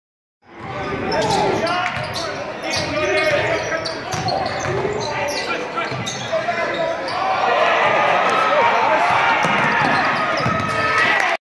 Iona Prep over Cardinal Hayes 85-74
Thomas Capuano of Iona Prep with a steal and fast-break layup to help Iona Prep hold off a 4th quarter run by Cardinal Hayes on Friday night in the Bronx.  Capuano had 23 points for Iona Prep. Box Score